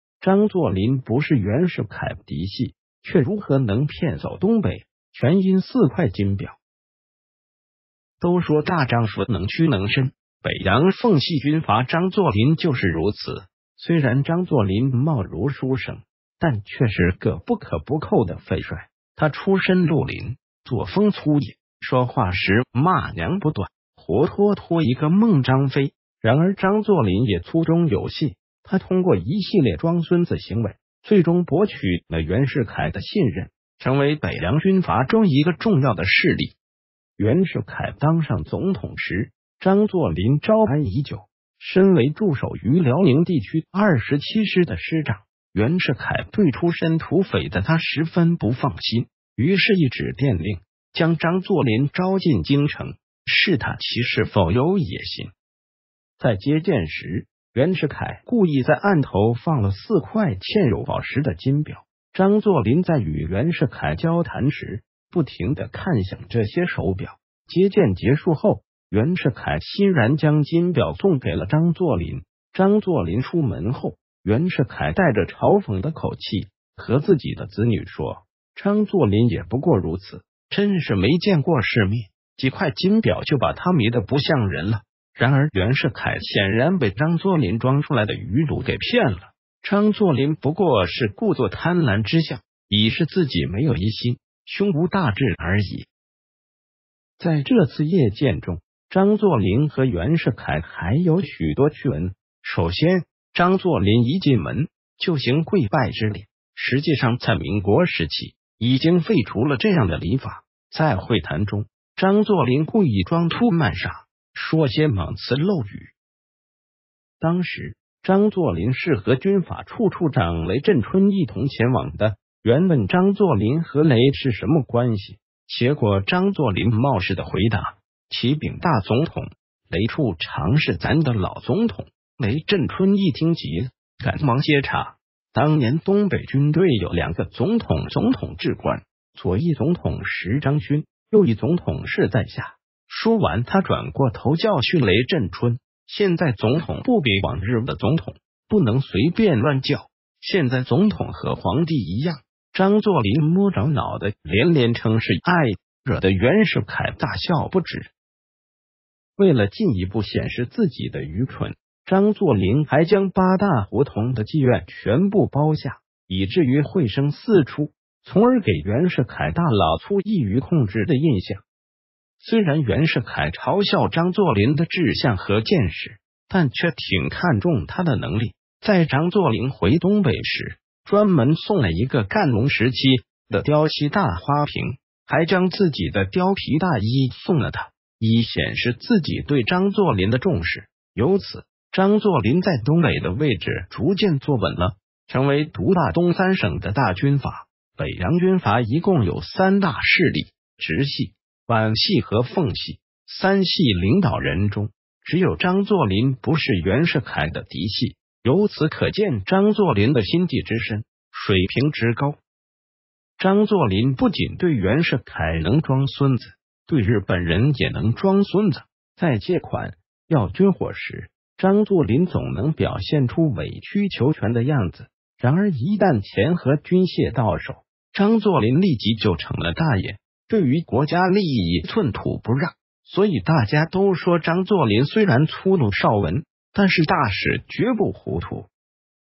张作霖不是袁世凯嫡系，却如何能骗走东北？全因四块金表
都说大丈夫能屈能伸，北洋奉系军阀张作霖就是如此。虽然张作霖貌如书生，但却是个不可不扣的“匪帅”。他出身绿林，作风粗野，说话时骂娘不断，活脱脱一个猛张飞。然而张作霖也粗中有细，他通过一系列“装孙子”行为，最终博取了袁世凯的信任，成为北洋军阀中一个重要的势力。

袁世凯当上总统时，张作霖招安已久，身为驻守于辽宁地区二十七师的师长。袁世凯对出身土匪的他十分不放心，于是一纸电令将张作霖招进京城，试探其是否有野心。
在接见时，袁世凯故意在案头放了四块嵌有宝石的金表。张作霖在与袁世凯交谈时，不停地看向这些手表，接见结束后，袁世凯欣然将金表送给了张作霖。张作霖出门后，袁世凯带着嘲讽的口气和自己的子女说：“张作霖也不过如此，真是没见过世面，几块金表就把他迷得不像人了。”然而袁世凯显然被张作霖装出来的愚鲁给骗了，张作霖不过是故作贪婪之像，以示自己没有异心，胸无大志而已。

在这次谒见中，张作霖和袁世凯还有许多趣闻。首先，张作霖一进门，就行跪拜之礼，实际上在民国时期，已经废除了这样的礼法。在会谈中，张作霖故意装粗卖傻，说些莽词漏语。
当时，张作霖是和军法处处长雷震春一同前往的，袁问张作霖和雷是什么关系。结果张作霖“冒失”地回答：“启禀大总统，雷处长是咱的老总统，”雷震春一听急了，赶忙接茬：“当年东北军队有两个总统（总统制官），左翼总统时张勋，右翼总统是在下。”说完他转过头教训雷震春：“现在总统不比往日的总统，不能随便乱叫，现在总统和皇帝一样！”张作霖摸着脑袋连连称是i，惹得袁世凯大笑不止。
为了进一步显示自己的愚蠢，张作霖还将八大胡同的妓院全部包下，以至于秽声四出，从而给袁世凯大老粗易于控制的印象。

虽然袁世凯嘲笑张作霖的志向和见识，但却挺看重他的能力。在张作霖回东北时，专门送了一个乾隆时期的雕漆大花瓶，还将自己的貂皮大衣送了他，以显示自己对张作霖的重视。由此，张作霖在东北的位置逐渐坐稳了，成为独霸东三省的大军阀。北洋军阀一共有三大势力，直系、皖系和奉系，三系领导人中，只有张作霖不是袁世凯的嫡系，由此可见张作霖的心计之身，水平之高。
张作霖不仅对袁世凯能装孙子，对日本人也能“装孙子”。在借款、要军火时，张作霖总能表现出委曲求全的样子，然而一旦钱和军械到手，张作霖立即就成了大爷，对于国家利益寸土不让。所以大家都说张作霖虽然粗鲁少文，但是大事绝不糊涂。